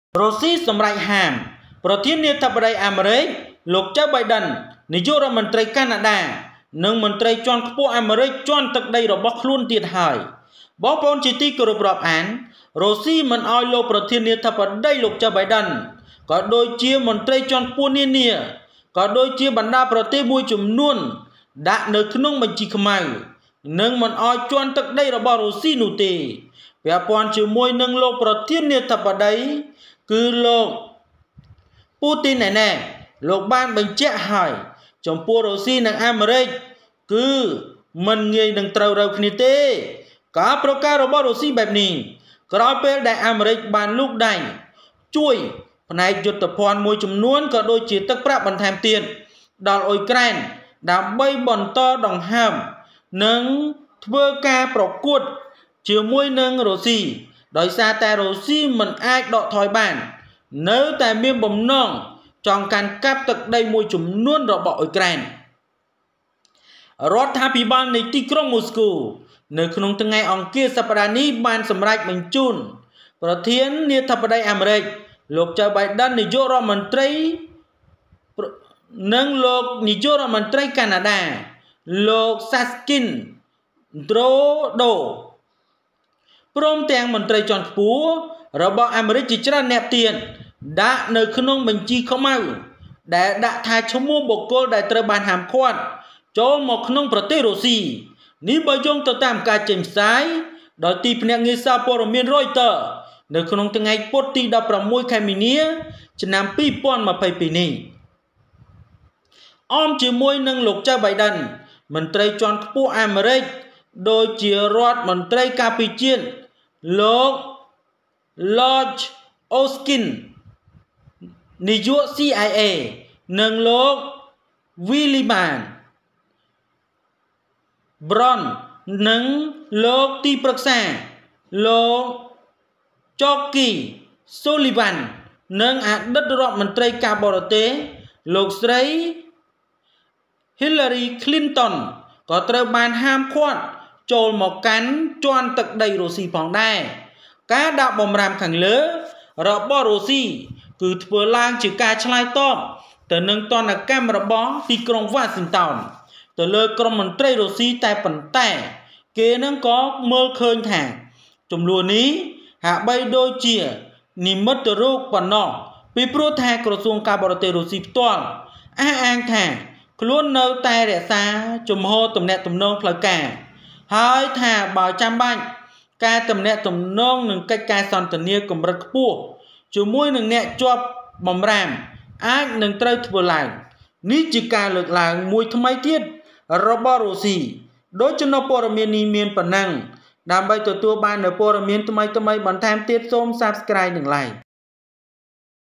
រុស្ស៊ី សម្រេចហាម លោក ចូ បៃដិន និងនាយករដ្ឋមន្ត្រីកាណាដា មន្ត្រីជាន់ខ្ពស់អាមេរិក ជាន់ទឹកដីរបស់ខ្លួនទេ
រុស្ស៊ី សម្រេចហាម លោក ចូ បៃដិន និងនាយករដ្ឋមន្ត្រីកាណាដា មន្ត្រីជាន់ខ្ពស់អាមេរិក ជាន់ទឹកដីរបស់ខ្លួនទេ Russia bans Joe Biden and Canadian PM from entering US.